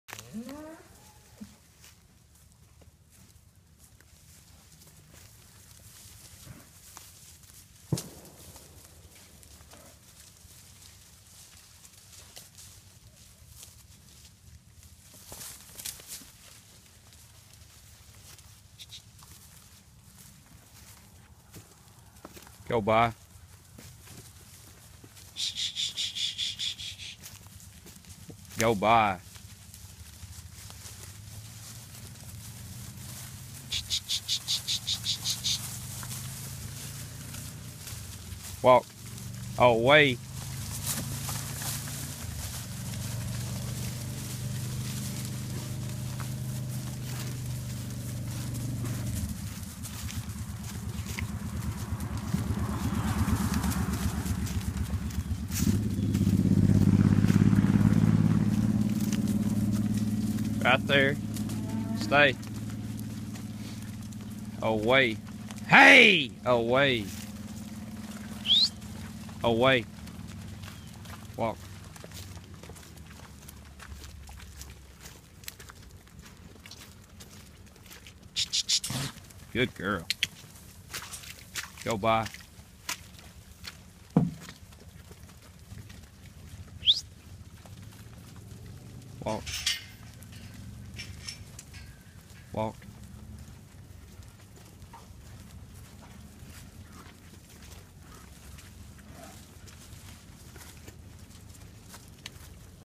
Sioux - Slash V - Take Pen - 7 wt Bulls
Sioux

Date: 12/30/2015

Slash V Bittersweet Charm
DOB: 1/27/2012
Small clip of Sioux taking some 6-7 bulls to the take pen to receive vaccinations.